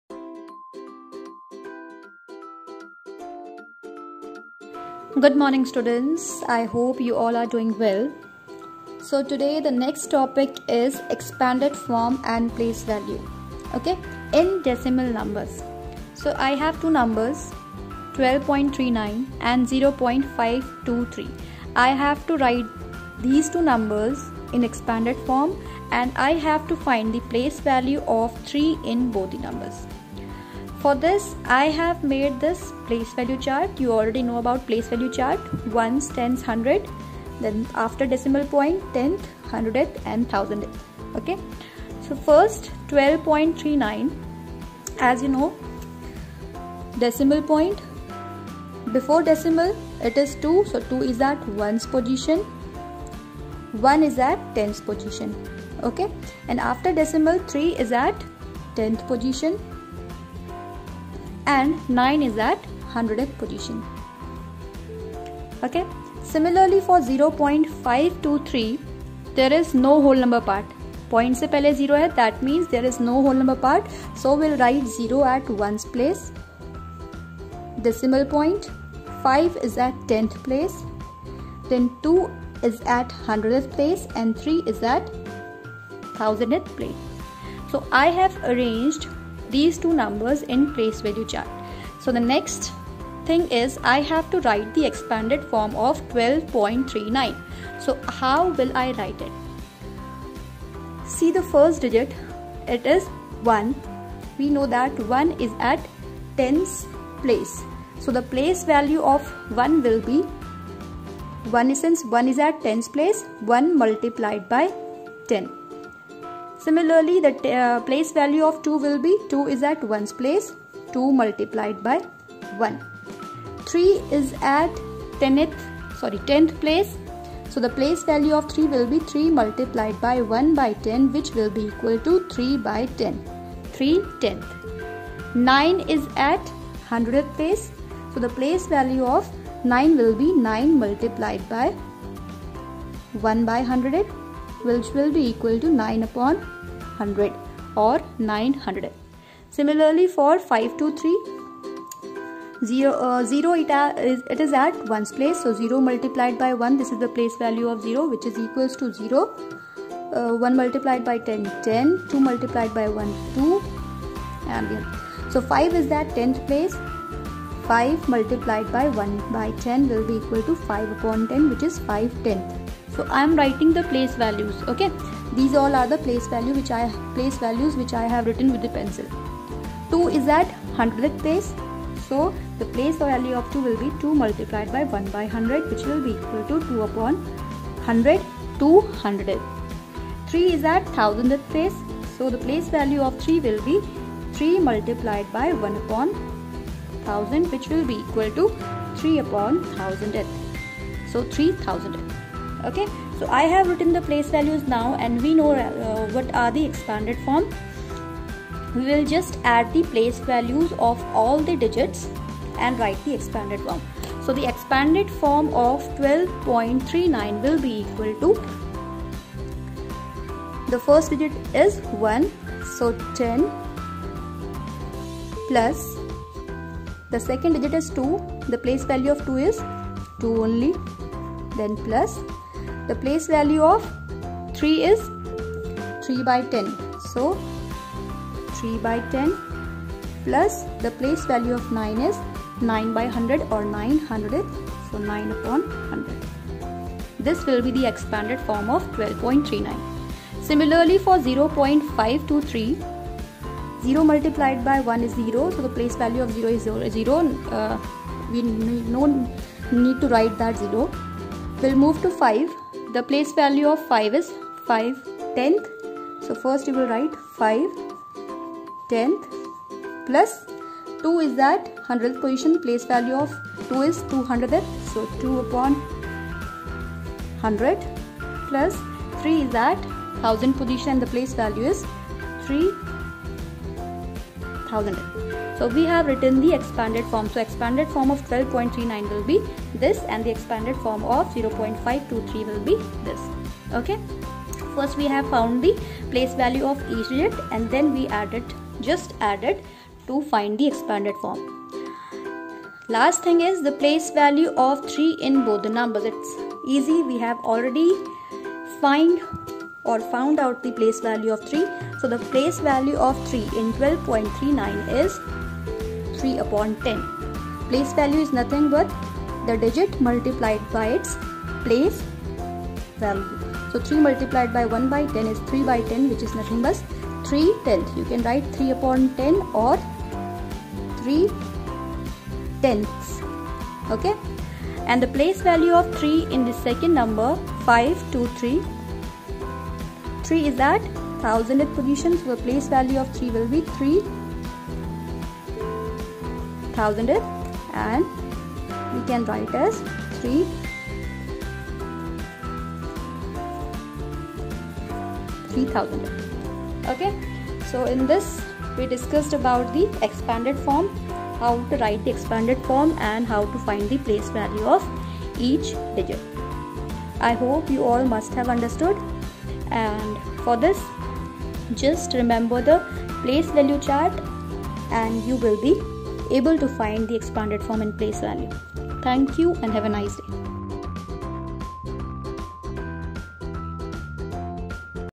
Expanded Form and place values of digits in decimal number ll Decimalsll CBSE CLASS 5
How to write the expanded form and find the place values of different digits in s Decimal Number.